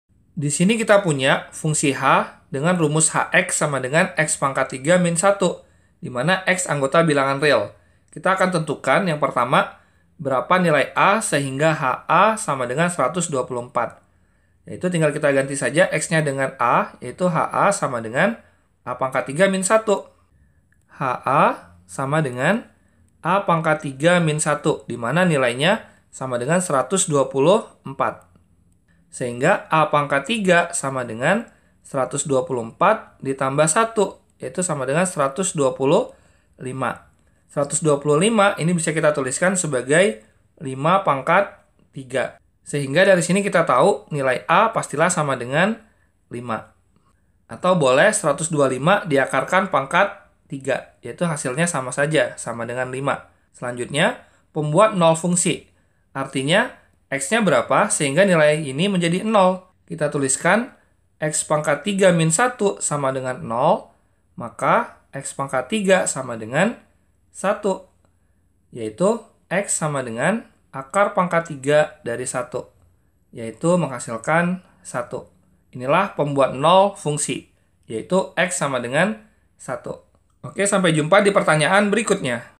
Fungsi h, didefinisikan dengan rumus h(x) = x? _1, dengan X & R, tentukanlah Bilangan a, hingga h...
Fungsi h, didefinisikan dengan rumus h(x) = x? _1, dengan X & R, tentukanlah Bilangan a, hingga h (a) = 124 a. Pembuat nol fungsi b_

Topik atau Materi: Nilai Fungsi - RELASI DAN FUNGSI - ALJABAR - Matematika Kelas 8 SMP - K-13

Sumber Soal: Matematika 2A Wajib kelas 10 K-13, EST 2016 , Penabur

00:00 : Bedah Soal
01:00 : Konsep, rumus dan pengertian Nilai Fungsi
01:18 : Langkah penyelesaian soal
00:22 : Jawaban akhir
00:52 : Penutup